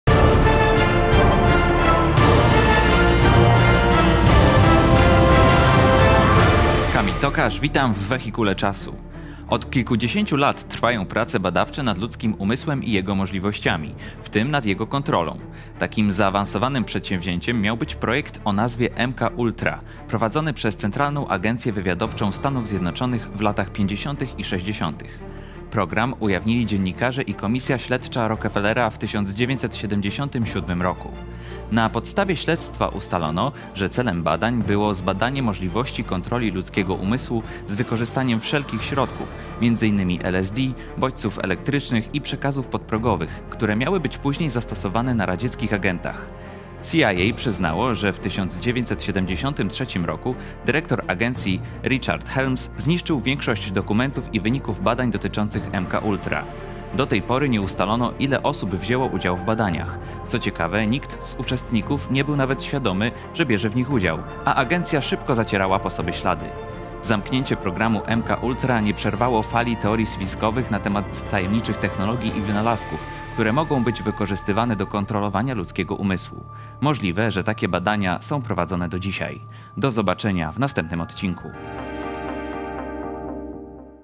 Chcieli kontrolować ludzkie umysły. Udało się im? - Wehikuł Czasu
Od kilkudziesięciu lat trwają prace nad ludzkim umysłem i jego możliwościami- w tym nad jego kontrolą. Takim zaawansowanym projektem miał być projekt o nazwie MKUltra, prowadzony przez Centralną Agencję Wywiadowczą Stanów Zjednoczonych w latach 50 i 60. Jak się zakończył? Zobacz najnowszy Wehikuł Czasu.